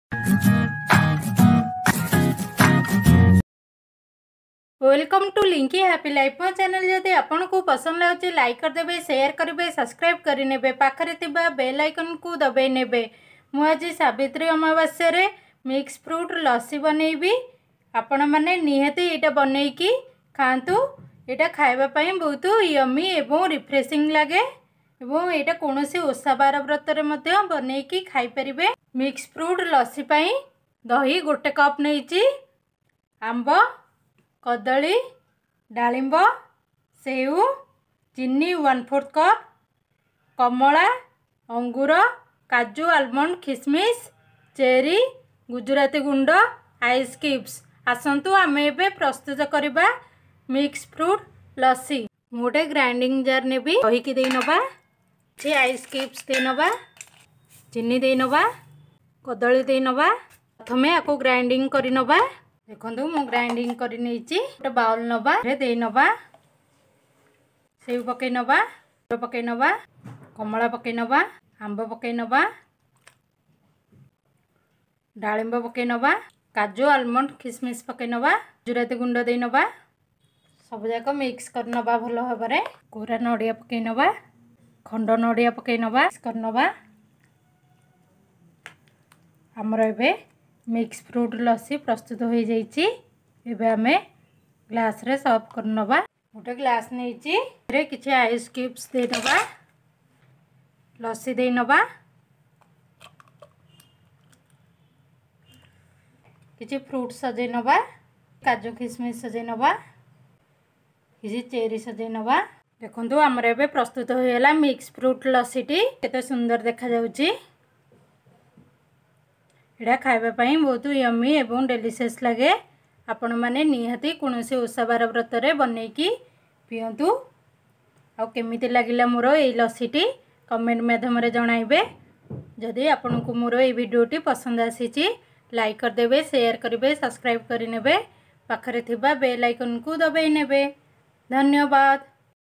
ସାବିତ୍ରୀ ବ୍ରତ ସ୍ପେଶାଲ ସ୍ଵାଦିଷ୍ଟ ମିକ୍ସ ଫଳ ଲସି। Easy & yummy 😋 Mix Fruits Lassi in odia.
infringement--
1. Curd-1cup
2. sugar -1/4cup
3. Banana -1
4. orange-1
5. Apple-1
6. Grapes
7. grated coconut, coconut prices
8. cadamom powder-1/2teaspoon
9. cherry
10. ice cubes
11. Cashew nuts,Almond, raisins